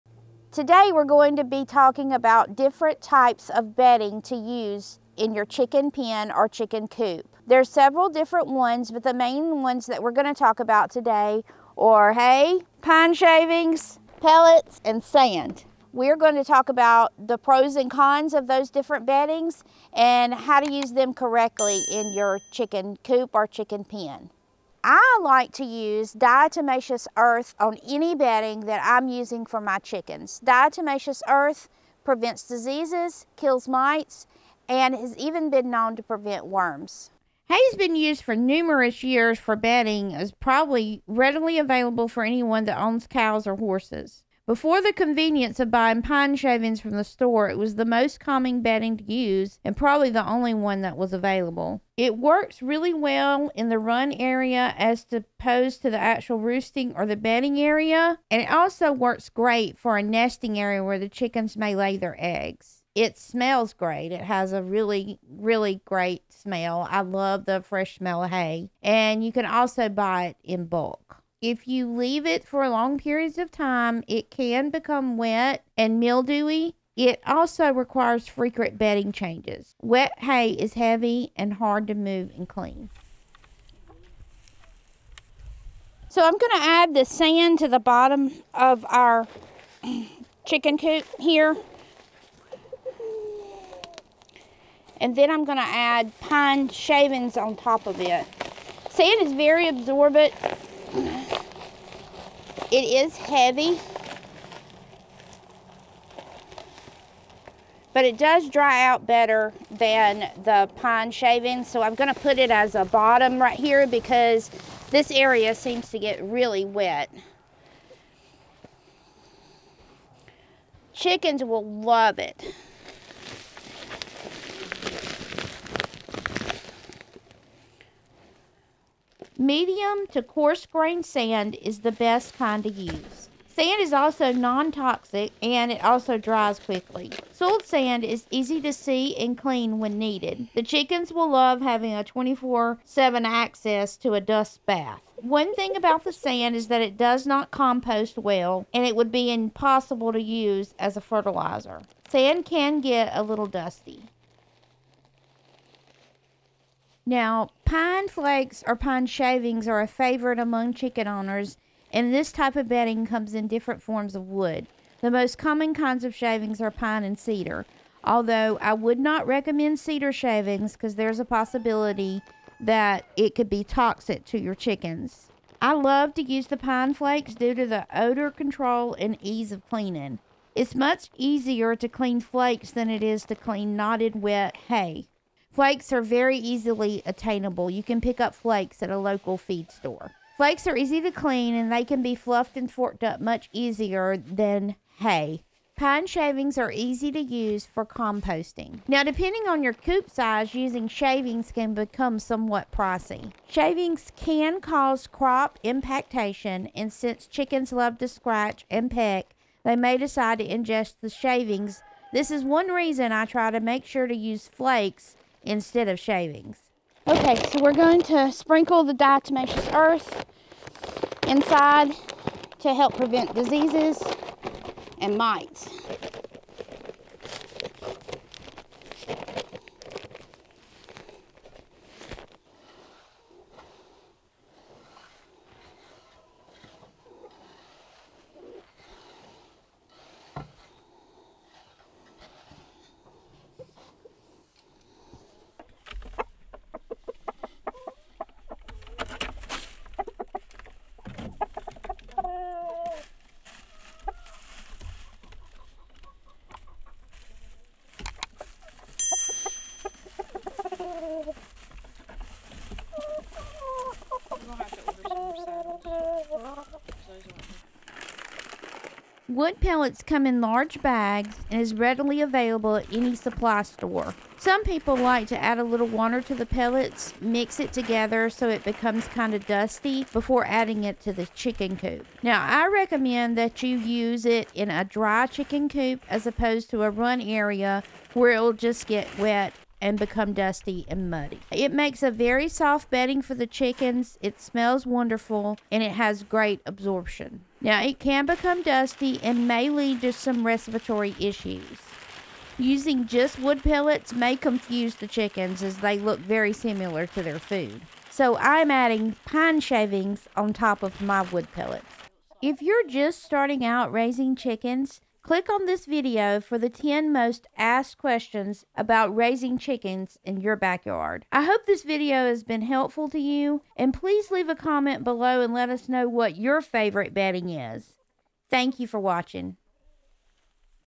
Chicken Coop Bedding Ideas for Beginners.
Making the decision about what type of bedding for chickens can be confusing at times, especially if you are just getting started raising chickens in your backyard. We will show you some of the most popular types of bedding used for the backyard chicken coop.

Types of materials used in this video include Hay, Sand, Pine Shavings and pellets.

Watch our video of the complete experience when we hatched chicken eggs for the first time;

Amazon link to the Manna Pro Harris Farms Nurture Right Incubator